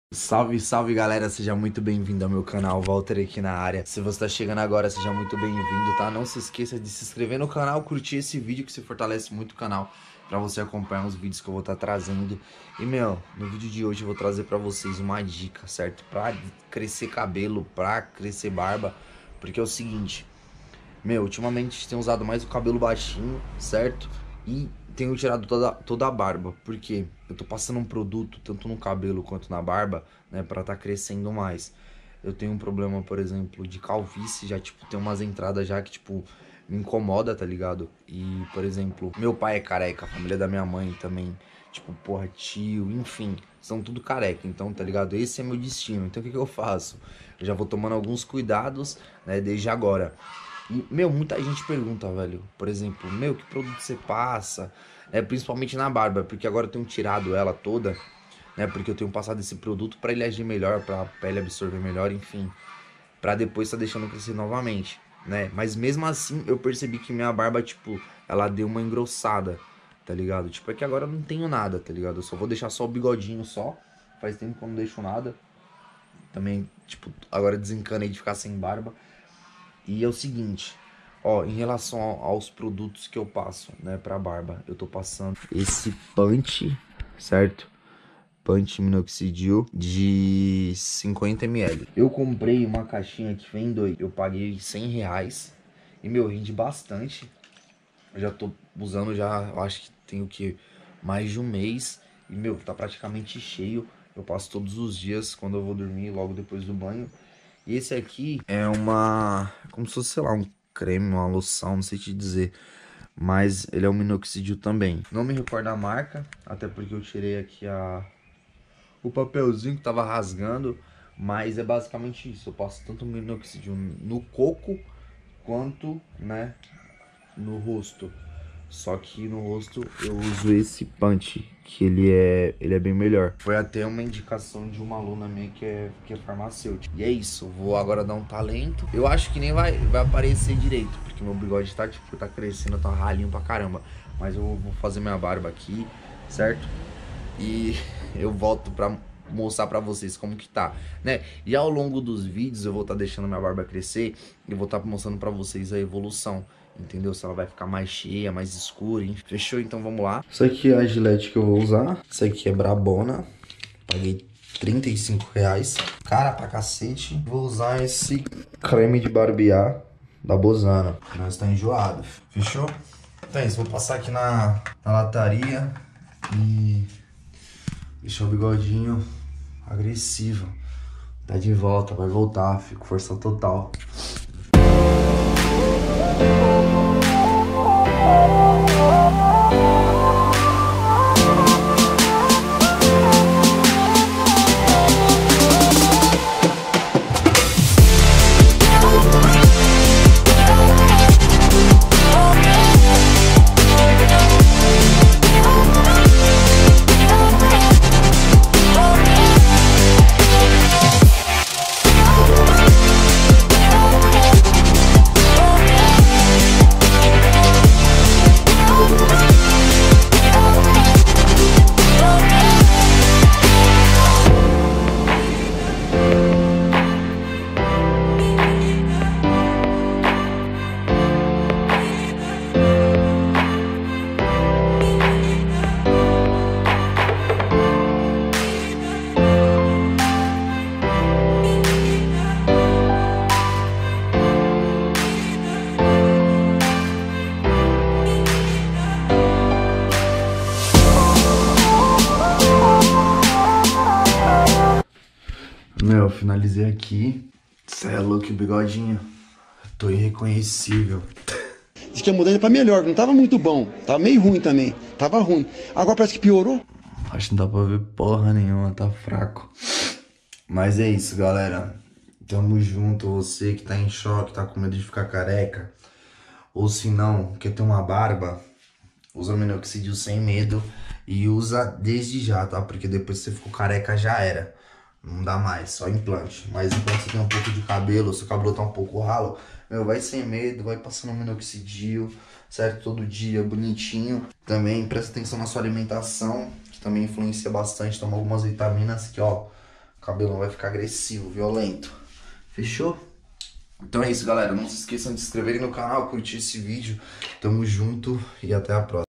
DICA PARA TER BARBA USANDO MINOXIDIL - LANCEI O BIGODE POLÊMICO 🤪
esse video mostro o efeito do minoxidil para ter barba, so que dessa vez vou deixar o bigode mais brabo que nunca, bigodinho polemico! KKK